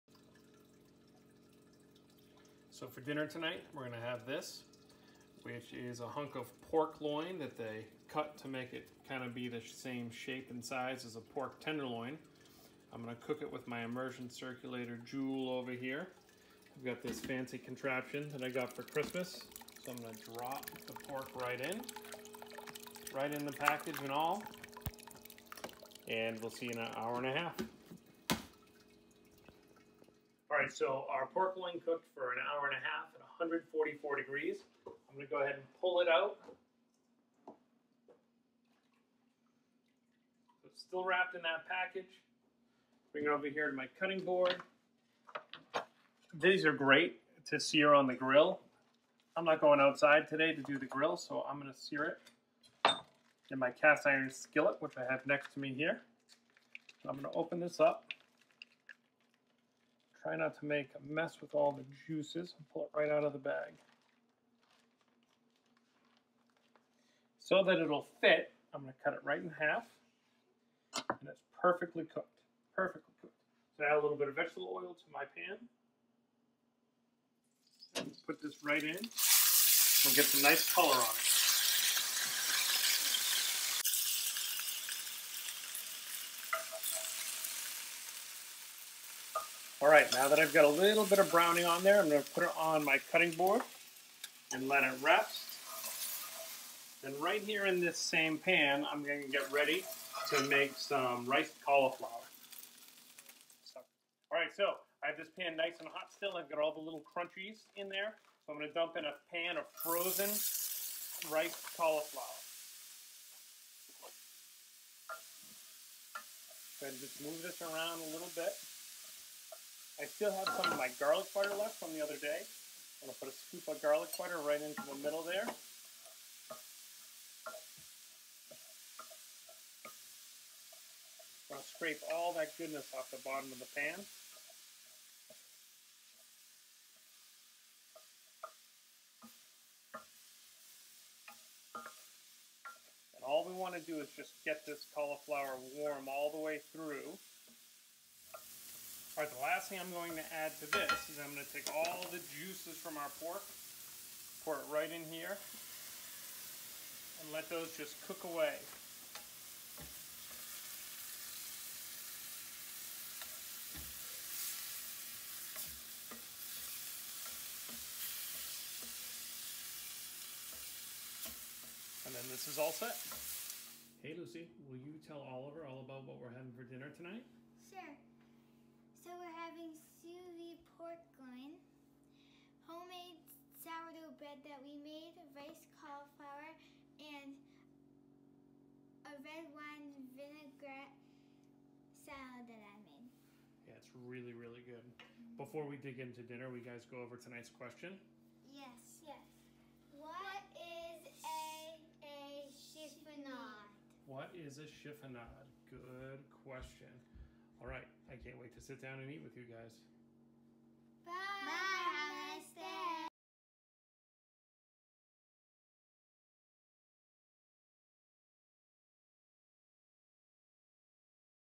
Sous Vide Pork Loin and Cauliflower Rice
We make a pretty simple (if you have an immersion circulator set up)  Sous Vide Pork Loin and Cauliflower Rice for dinner one night.